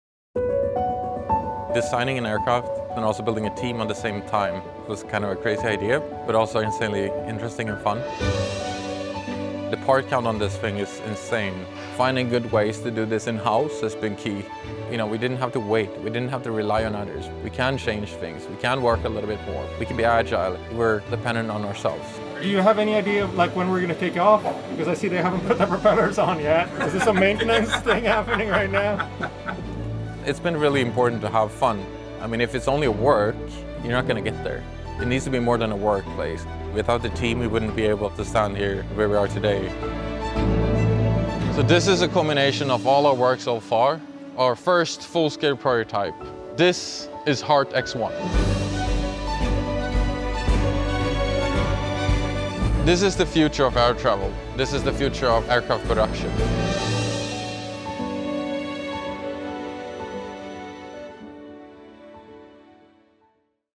Heart Aerospace, Heart X1 - How We Made It
Built almost entirely in-house at Heart’s Gothenburg facilities, the Heart X1 demonstrator reflects the company’s strategy to simultaneously develop both the design and production processes.